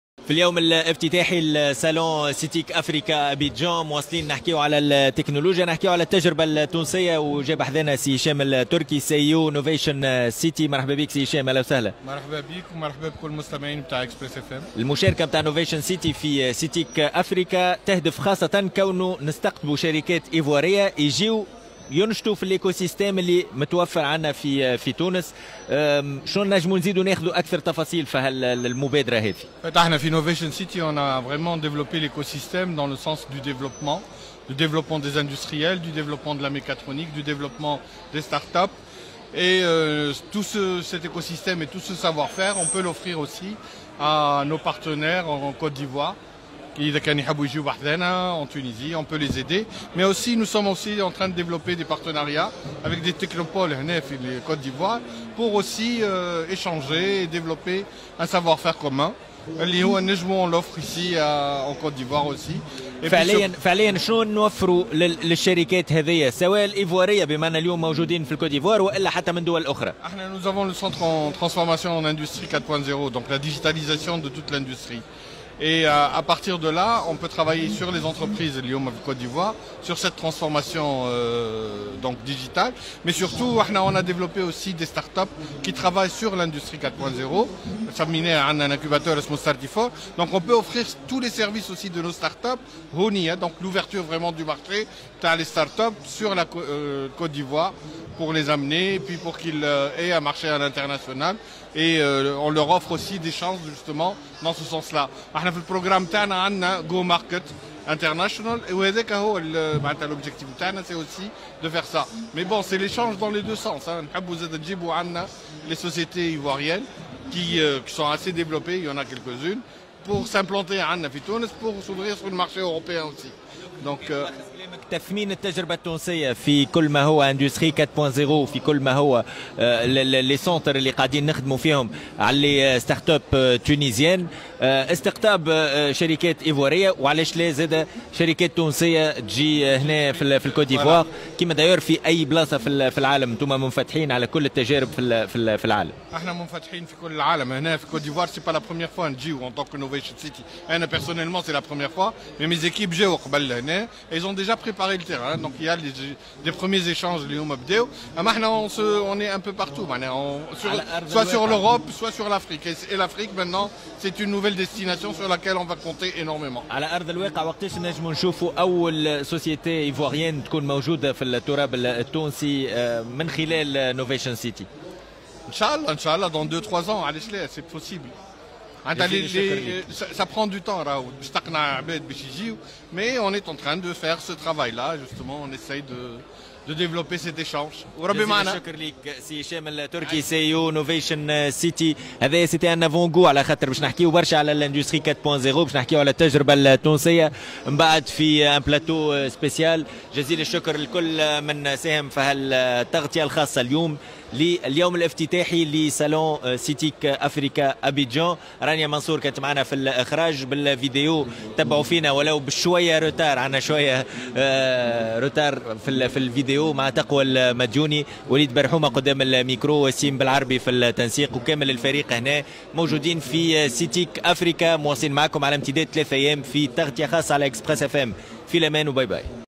Ouverture de marches pour les startups et attraction d'entreprises ivoiriennes en Tunisie
M. Hichem Turki CEO Novation City. Ouverture de marches pour les startups et attraction d'entreprises ivoiriennes en Tunisie en les intégrant dans l'écosystème Novation City